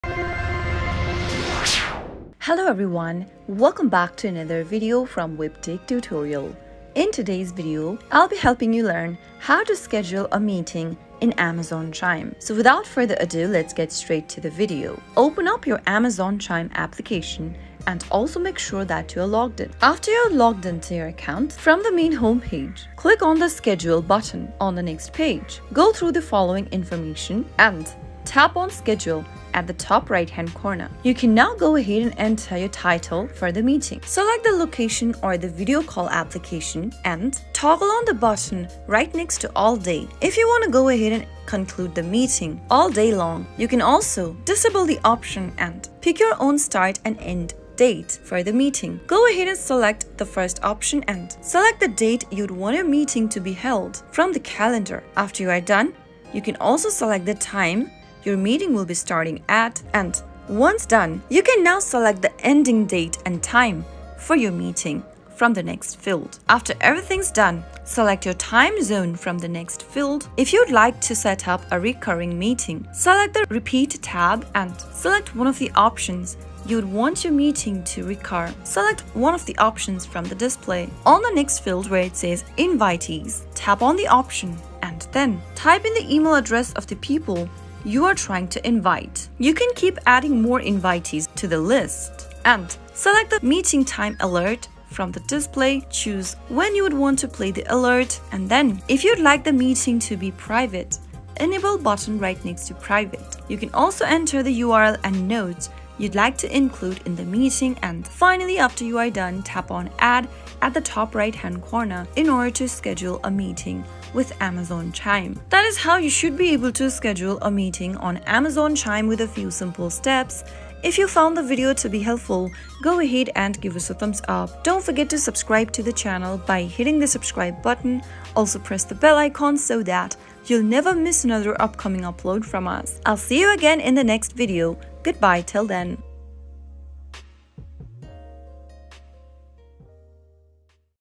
How To Schedule a Meeting in Amazon Chime 2023?
Welcome to our comprehensive tutorial on scheduling meetings in Amazon Chime! In this step-by-step guide, we'll walk you through the process of effortlessly setting up and managing meetings using Amazon Chime's powerful features. Discover how to streamline your collaboration, ensure prompt attendance, and maximize productivity. Join us as we unlock the full potential of Amazon Chime's meeting scheduling capabilities!